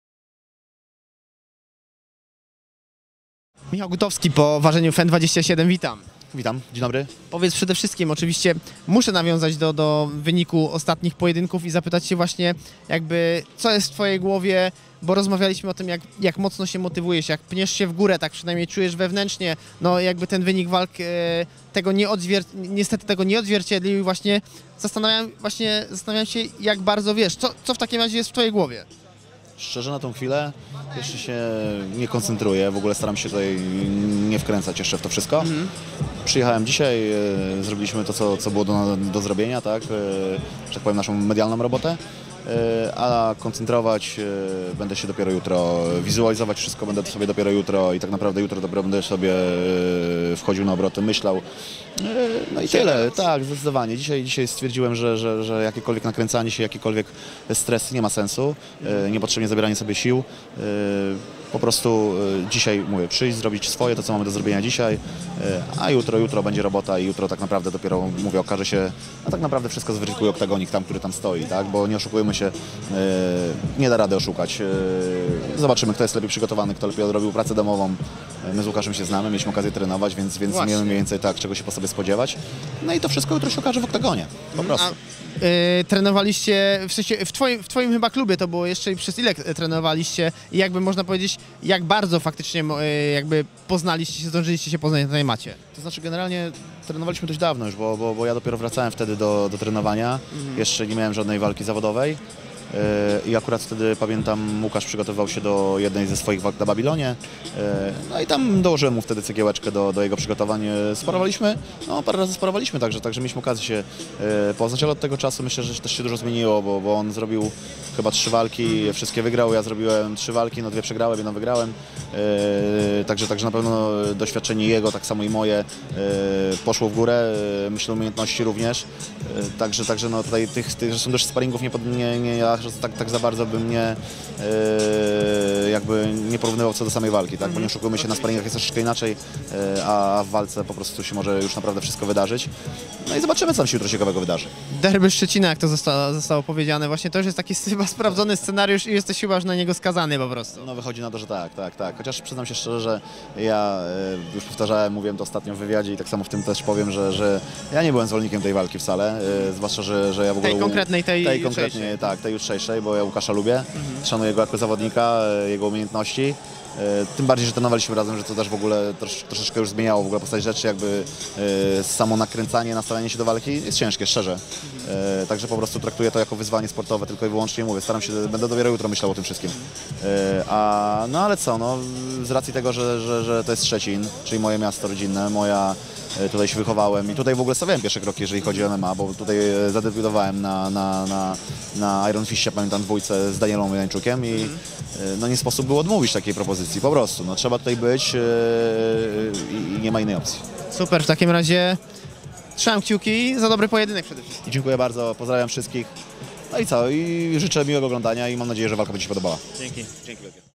Michał Gutowski nie jest zwolennikiem walki „z kolegą” Sudolskim na FEN 27
Okazuje się, że walka Michała Gutowskiego z Łukaszem Sudolskim jest o tyle trudna, że łączą ich koleżeńskie więzy. Tym samym ciężko jest wejść w odpowiedni tryb nastawienia psychicznego.